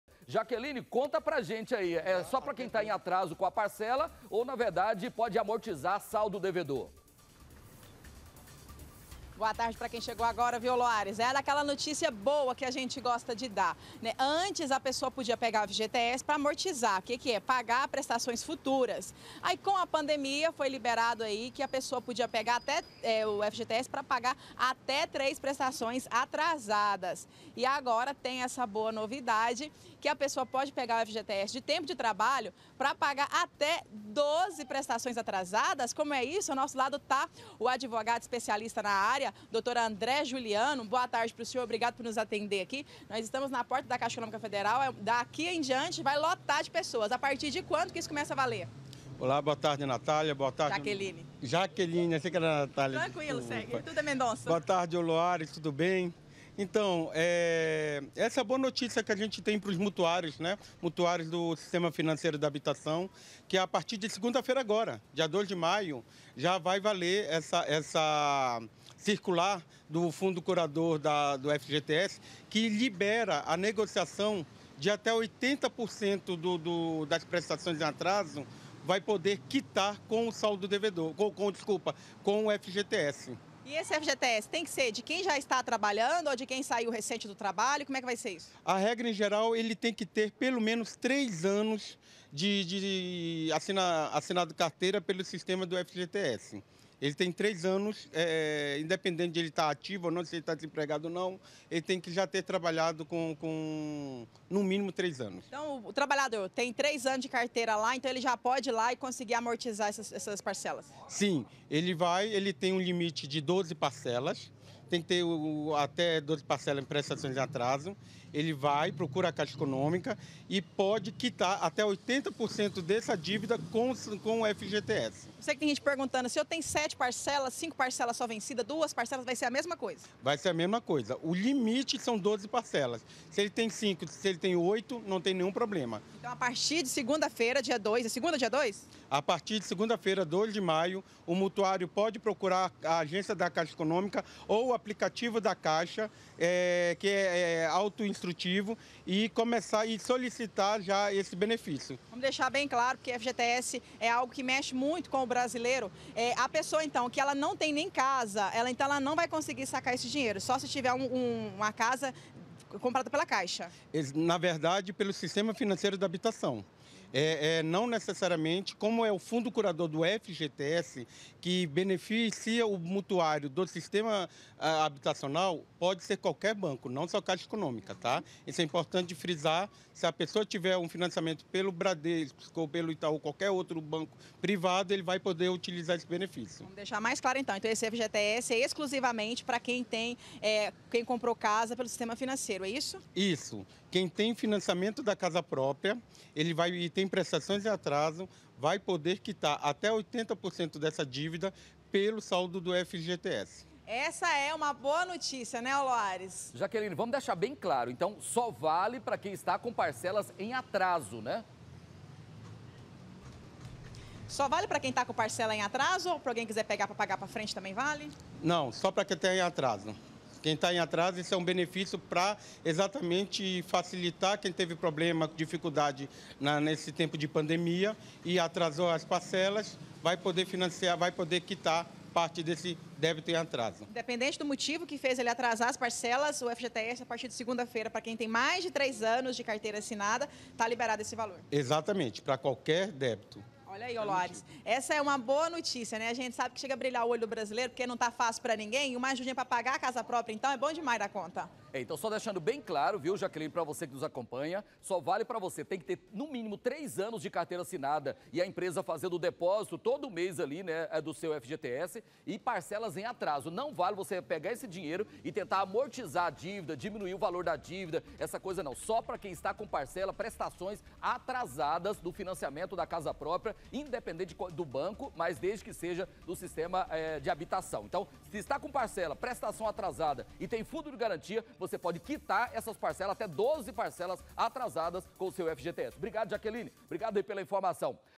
ATÉ 12 PARCELAS ATRASADAS: FGTS PODERÁ SER USADO PARA QUITAR CASA PRÓPRIA
FGTS PODERÁ SER USADO PARA QUITAR ATÉ 12 PARCELAS ATRASADAS DA CASA PRÓPRIA.